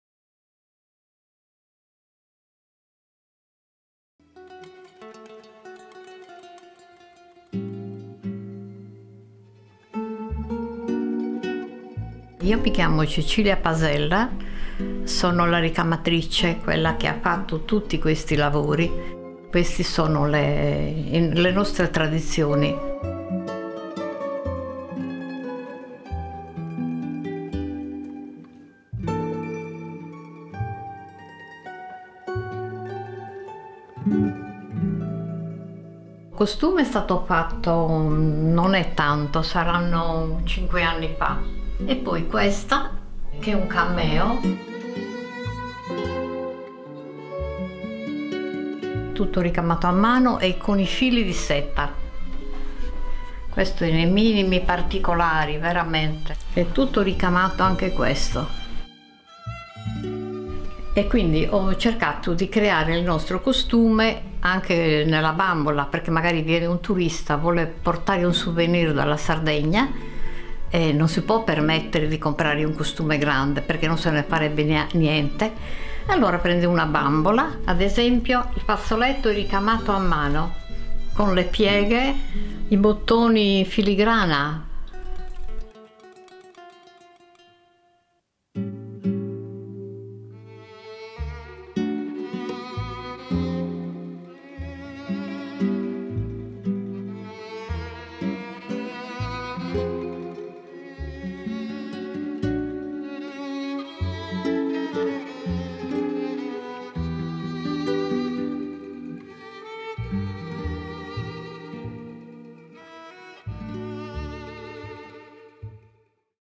Traditionelle sardische Trachten ENTDECKE SARDINIEN
Sardische Trachten und ein Umzug in Siniscola.